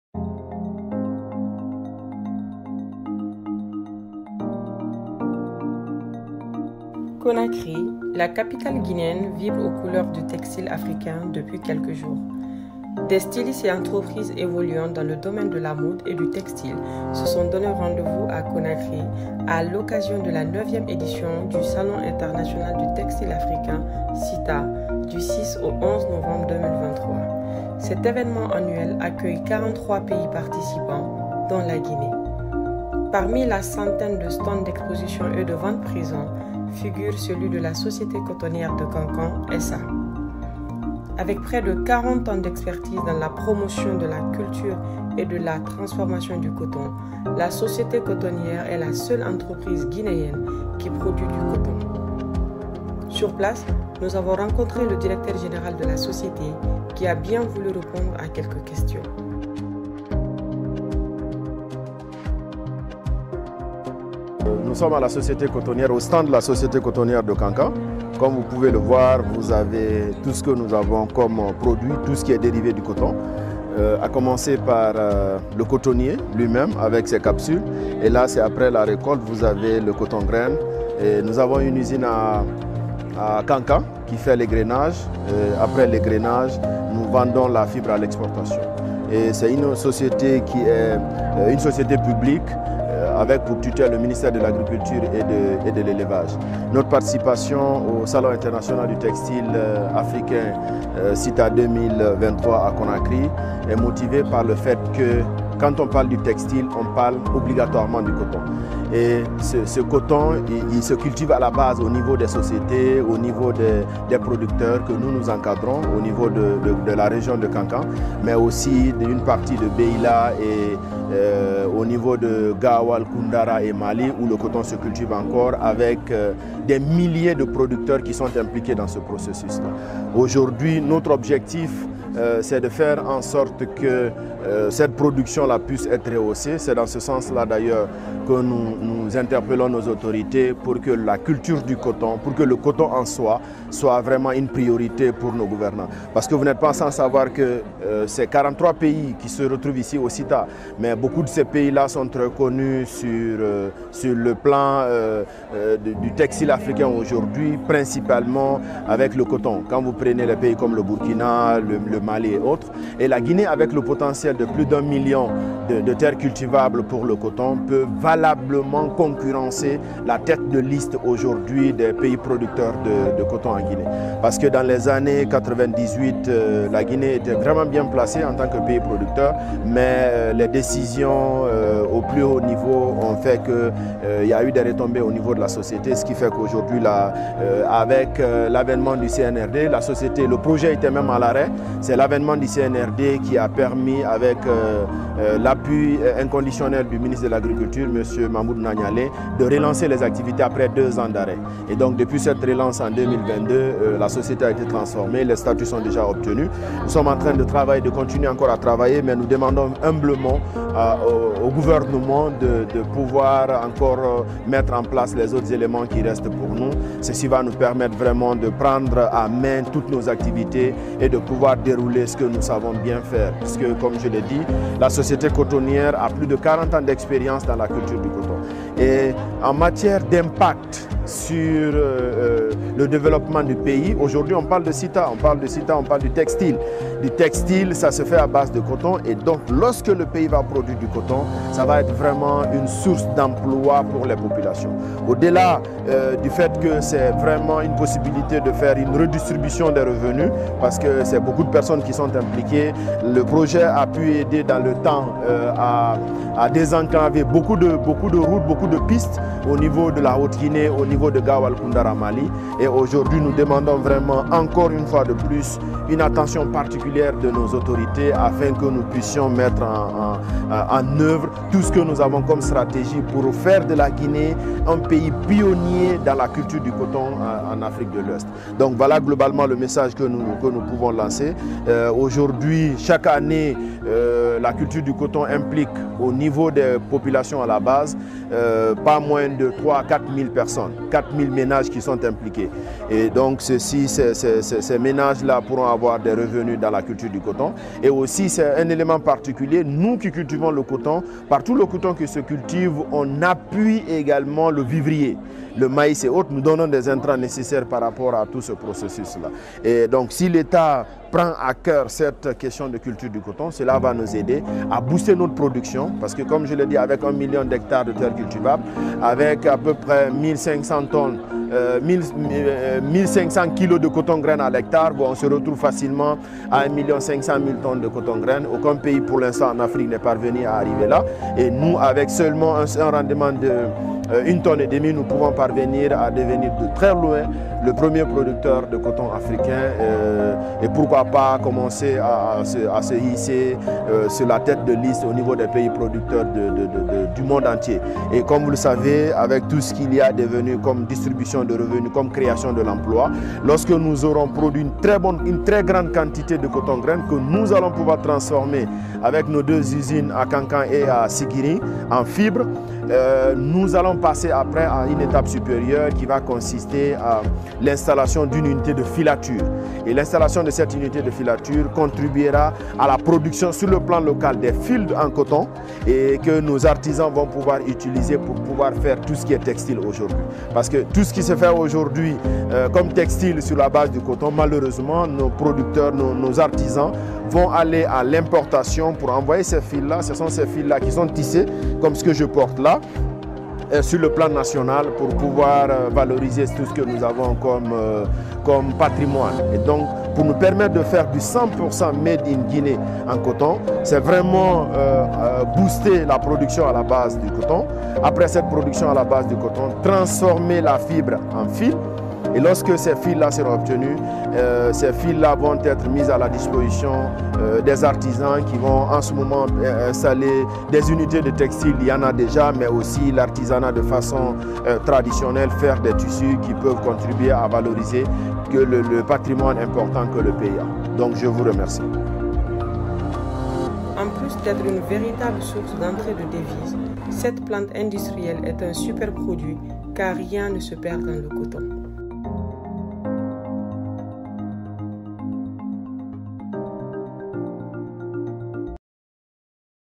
La Société Cotonnière de Kankan SA, partenaire du SITA Conakry .2023
Sous la tutelle du Ministère de l’Agriculture et de l’Elevage, la Société Cotonnière de Kankan SA (SCK SA) est une entreprise publique qui évolue dans la promotion et la commercialisation du coton.

Du 06 au 11 novembre, la SCK SA a pris part à au SITA Conakry 2023.

Reportage réalisé par le Service Communication et Relations Publiques.